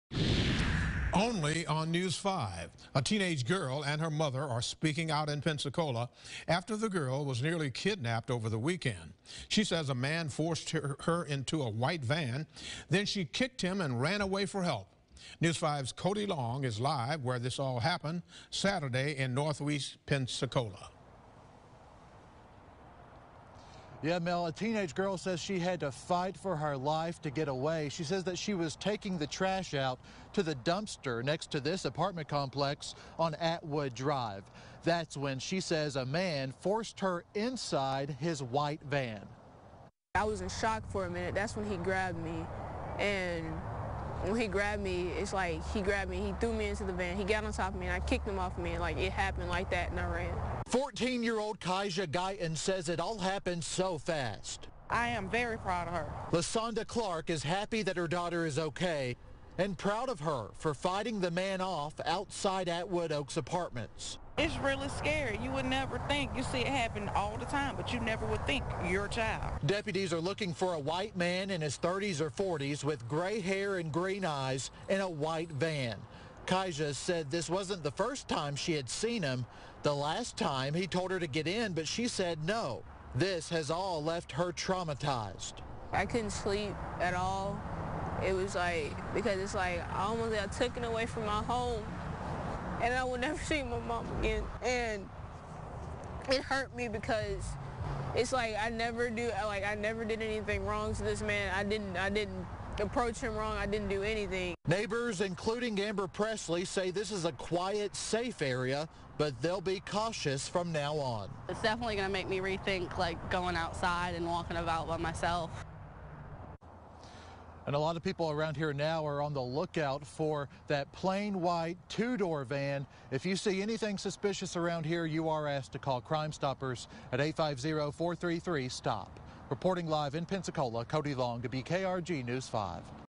VIDEO: Daughter, mother talk after attempted kidnapping in Pensacola
A 14-year-old girl is speaking about the terrifying moments she was almost kidnapped in Pensacola over the weekend.